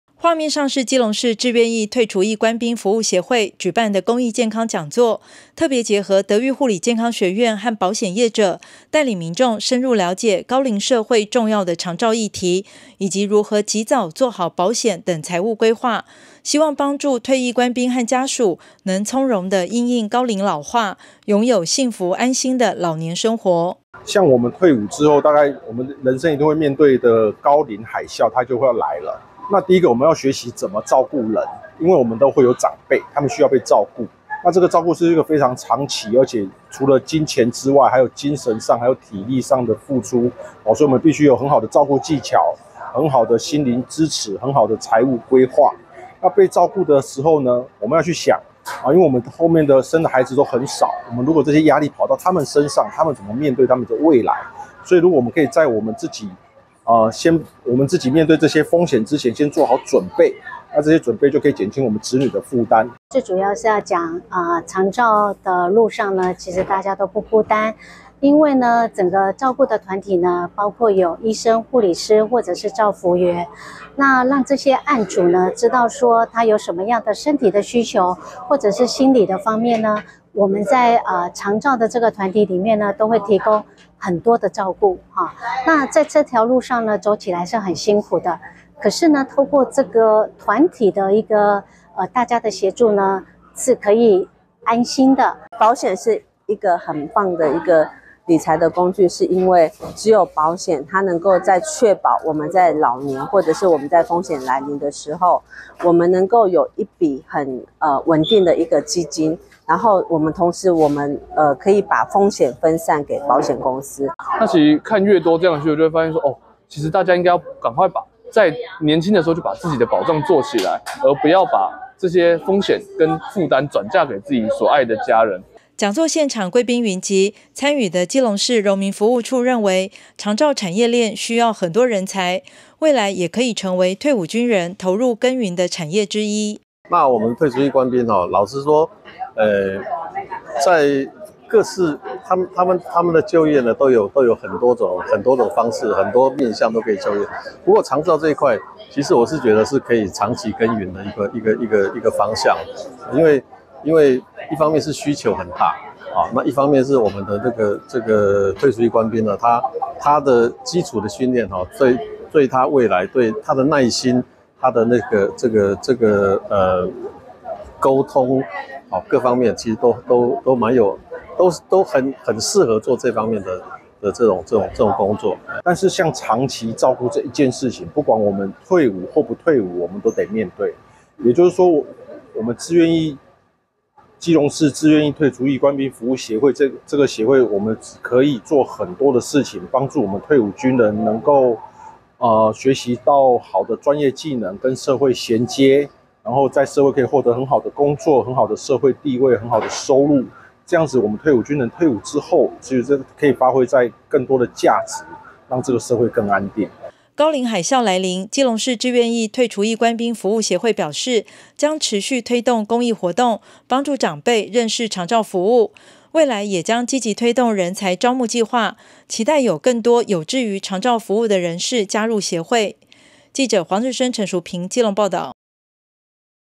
基隆市志願役退除役官兵服務協會 舉辦長照公益講座
畫面上是基隆市志願役退除役官兵服務協會舉辦的公益健康講座，特別結合德育護理健康學院和保險業者，帶領民眾深入了解高齡社會重要的長照議題，以及如何及早做好保險等財務規劃，希望幫助退役官兵和家屬能從容的因應高齡老化，擁有幸福安心的老年生活。

講座現場貴賓雲集，參與的基隆市榮民服務處也觀察到長照產業鏈需要很多人才，未來也可成為退伍軍人貢獻力量投入耕耘的產業之一。

基隆市志願役退除役官兵服務協會理事長江志偉強調，希望透過講座，讓長者認識長照服務和這個產業，也做好相關退休金的財務規畫，面對高齡海嘯來臨，基隆市志願役退除役官兵服務協會將持續推動公益活動幫助長輩認識長照服務，未來也將積極推動人才招募計畫，期待更多有志於長照服務的人士加入協會。